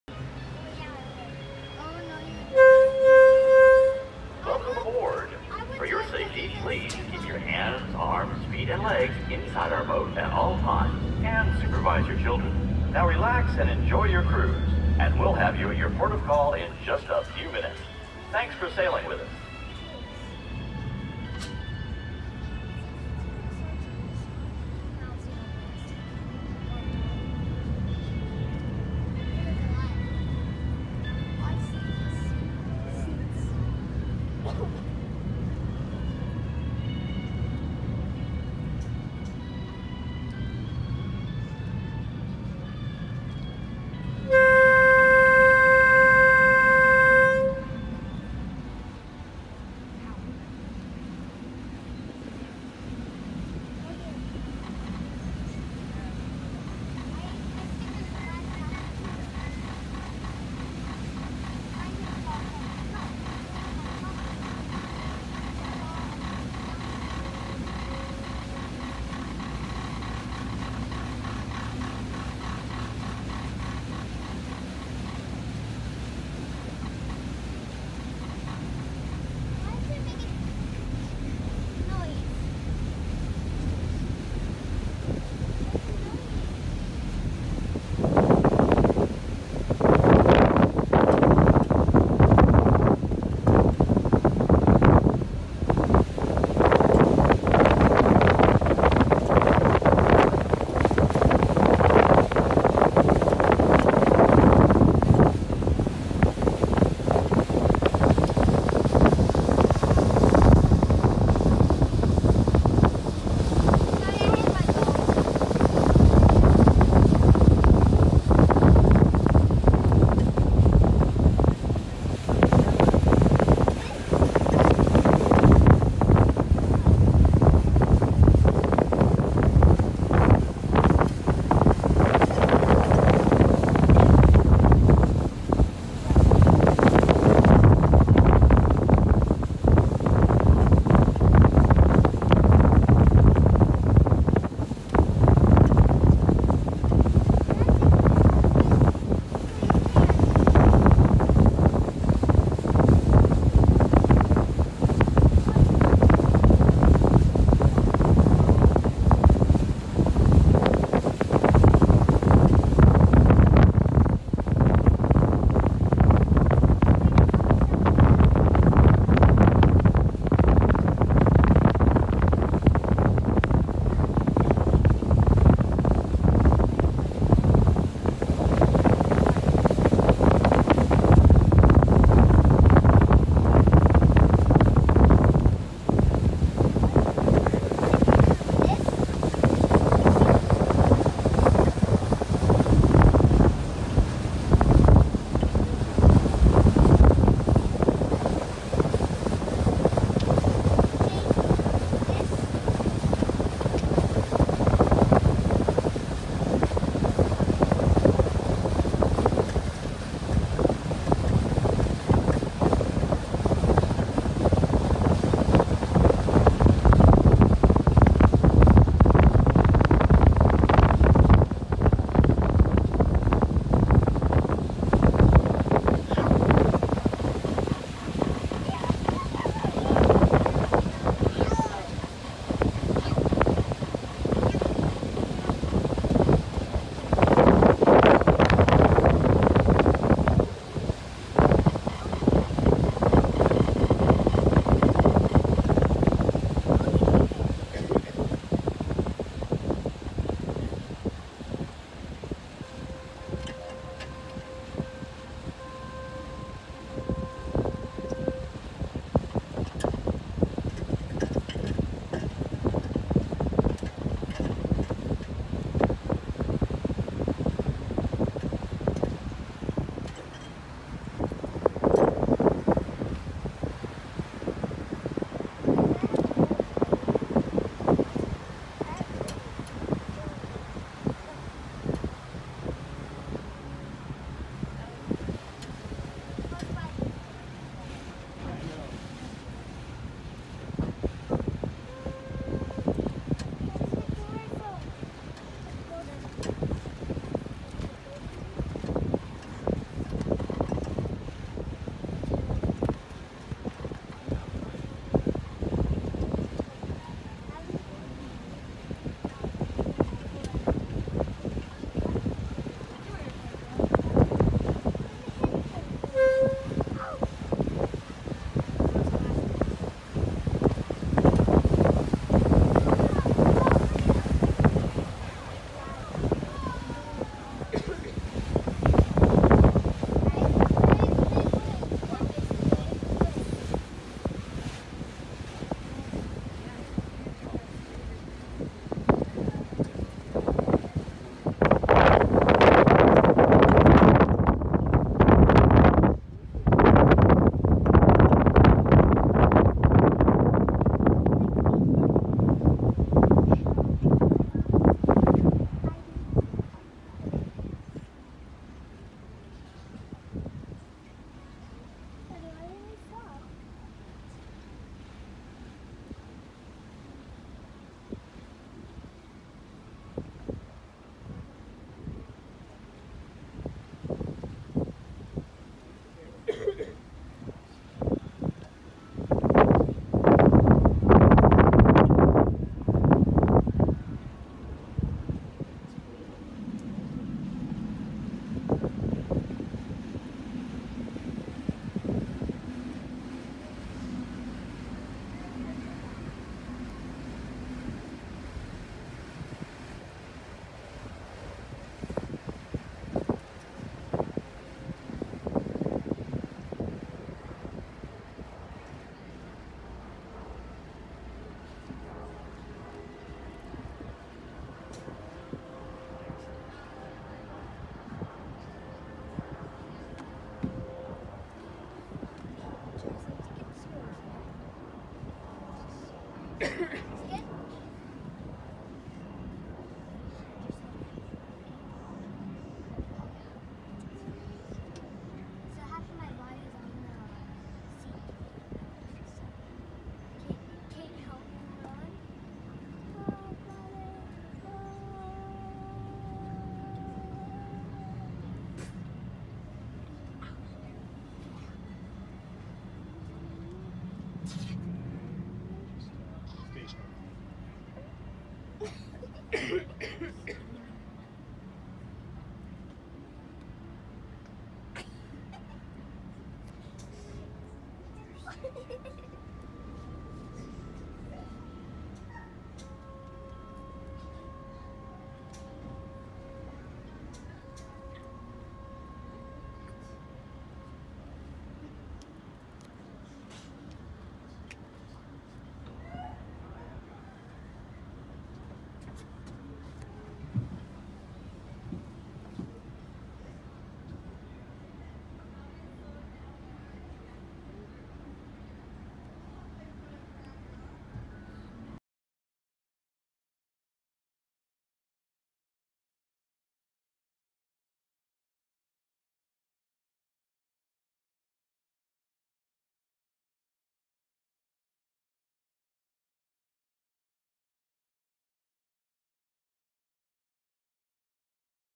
WDW Cruise from Magic Kingdom to Bay Lake
On one of our visits to Walt Disney World, we took an afternoon boat ride from the Magic Kingdom to Ft. Wilderness Lodge.  It was a relaxing ride to dinner after a long fun day at Magic Kingdom.  We had the front row and awesome views along the way.